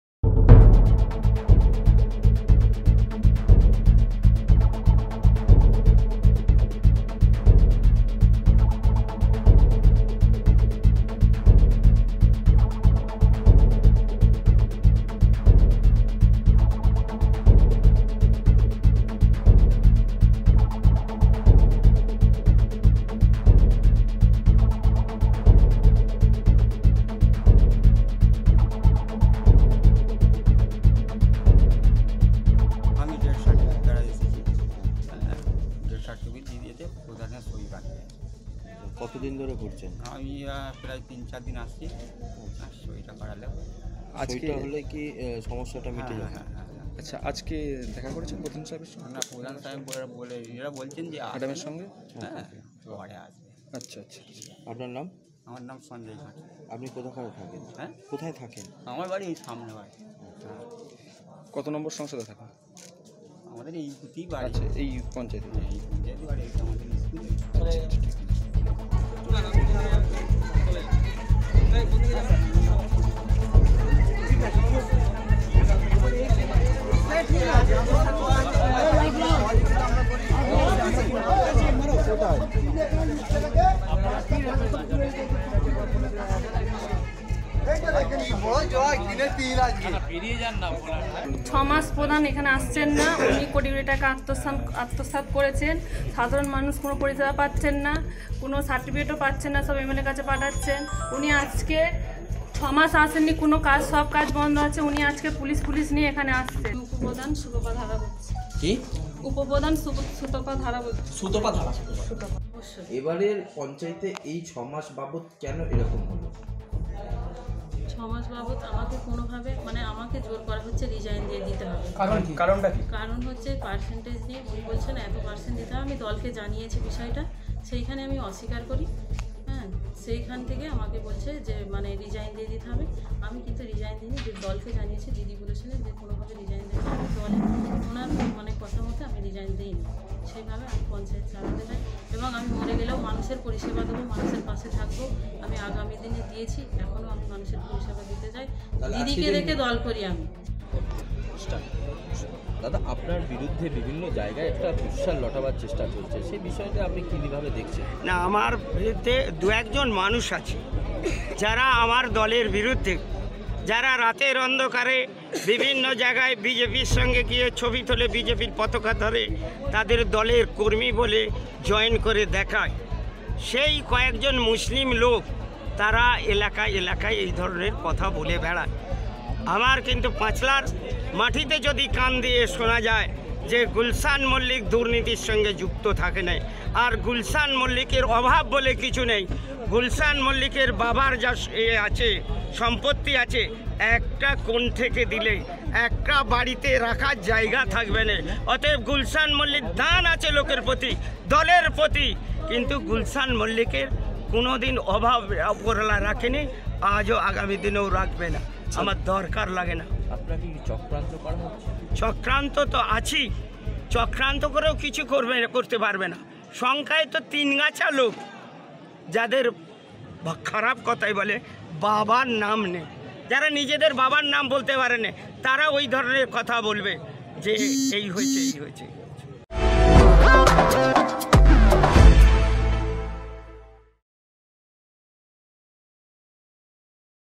টাইমস্ বাংলার অন্তর্তদন্ত পর্ব ১ - বিধায়ক-পঞ্চায়েত প্রধান দ্বন্ধে চড়ান্ত অবহেলিত নাগরিক পরিষেবা
রাকেশ মাইতি, হাওড়াঃ বিগত ৬ মাস অনুপস্থিত পঞ্চায়েত প্রধান, একাধিক দূর্নীতির অভিযোগ প্রধানের বিরুদ্ধে, বিধায়কের বিরূদ্ধে পাল্টা অভিযোগ প্রধানের